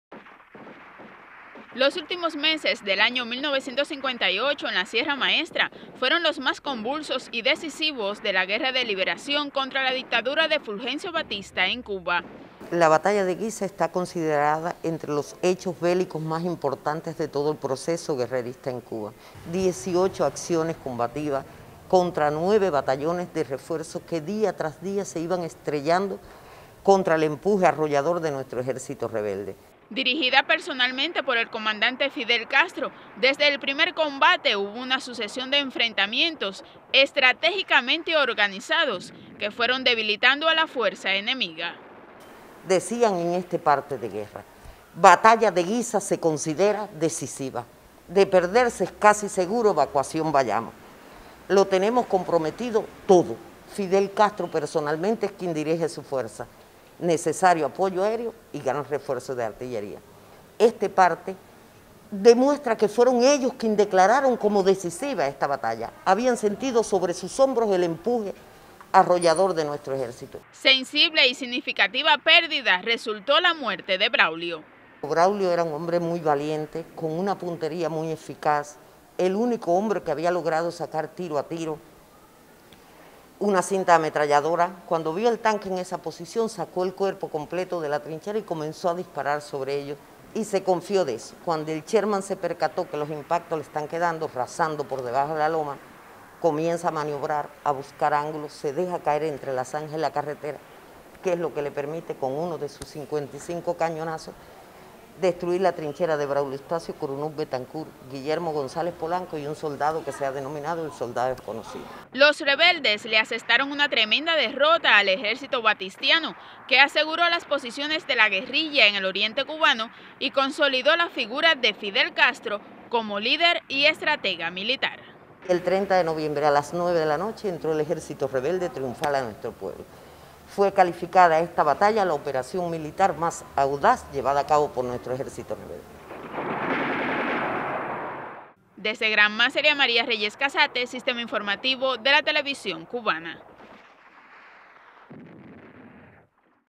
La batalla de Guisa
El 20 de noviembre de 1958 comenzó la histórica batalla de Guisa que culminó diez días después con una derrota total para la dictadura batistiana. El combate lo dirigió personalmente el Comandante en Jefe Fidel Castro  y fue una victoria esencial en la estrategia del Ejército Rebelde.